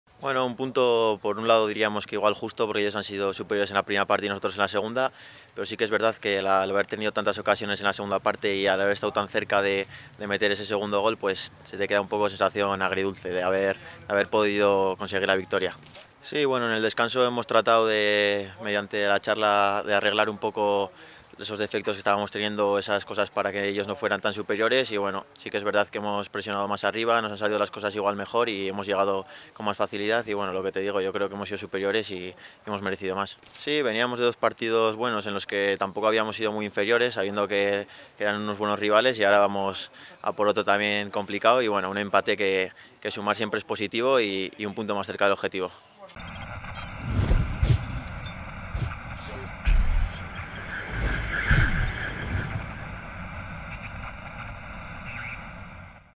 Moncayola: "Ellos han sido superiores en la primera parte y nosotros en la segunda".
Declaraciones de Moncayola tras el encuentro disputado entre Osasuna y Huesca en El Sadar (1-1).